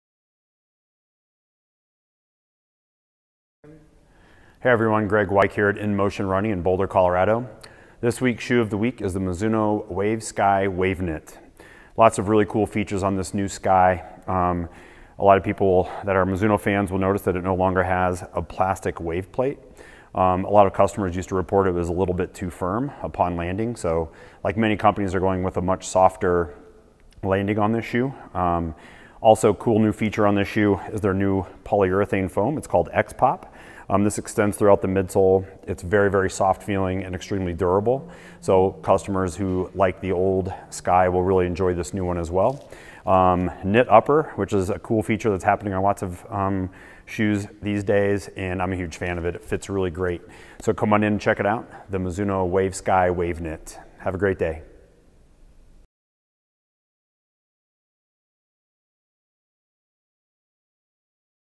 Shoe of the Week: Mizuno Wave Sky Waveknit 3
In Motion Running shoe buyer Greg Weich highlights the great new changes to this soft and comfortable neutral training shoe.